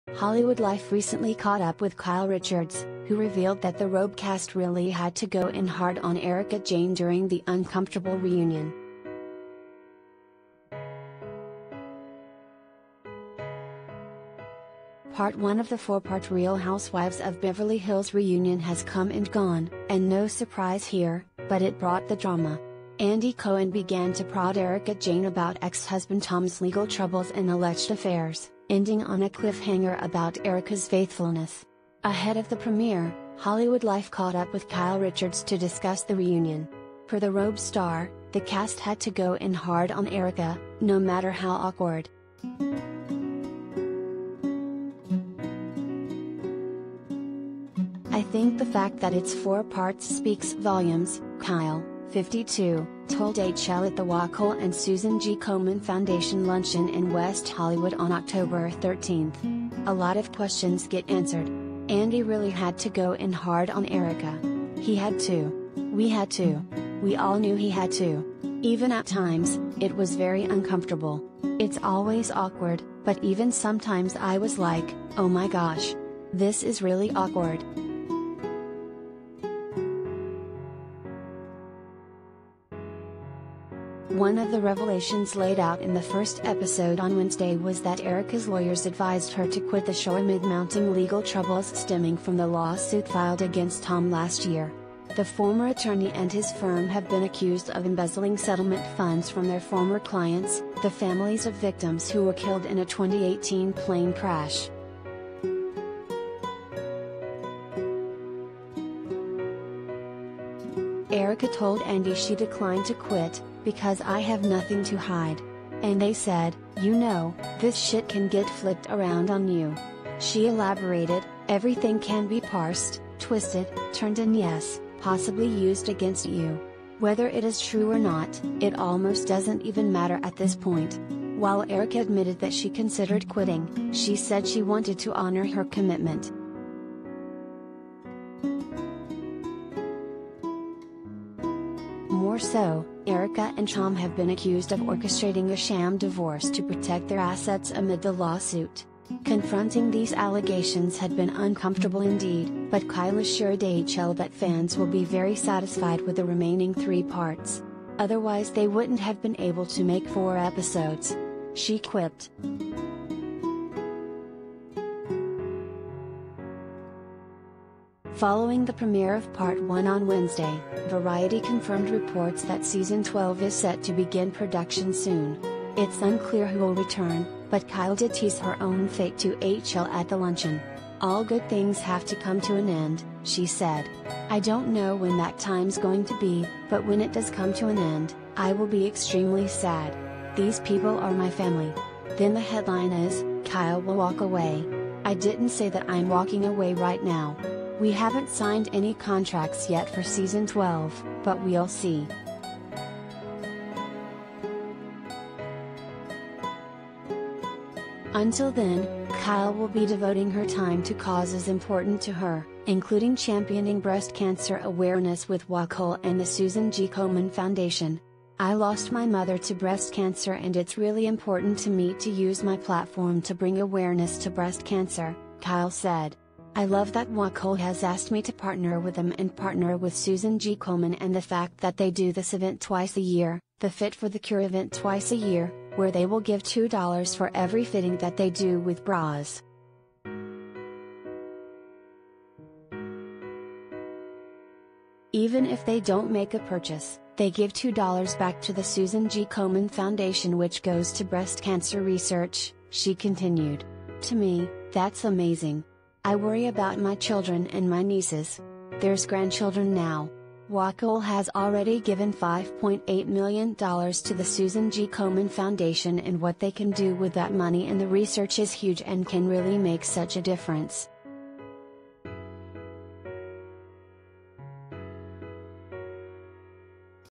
Kyle Richards Says ‘RHOBH’ Cast ‘Had To Go In Hard On’ Erika Jayne During Reunion
HollywoodLife recently caught up with Kyle Richards, who revealed that the ‘RHOBH’ cast ‘really had to go in hard’ on Erika Jayne during the ‘uncomfortable’ reunion.
Part one of the four-part Real Housewives of Beverly Hills reunion has come and gone — and no surprise here, but it brought the drama. Andy Cohen began to prod Erika Jayne about ex-husband Tom‘s legal troubles and alleged affairs, ending on a cliffhanger about Erika’s faithfulness. Ahead of the premiere, HollywoodLife caught up with Kyle Richards to discuss the reunion. Per the RHOBH star, the cast “had to go in hard” on Erika, no matter how awkward.
“I think the fact that it’s four parts speaks volumes,” Kyle, 52, told HL at the Wacoal and Susan G. Komen Foundation Luncheon in West Hollywood on October 13. “A lot of questions get answered. Andy really had to go in hard on Erika. He had to. We had to. We all knew he had to. Even at times, it was very uncomfortable. It’s always awkward, but even sometimes I was like, ‘O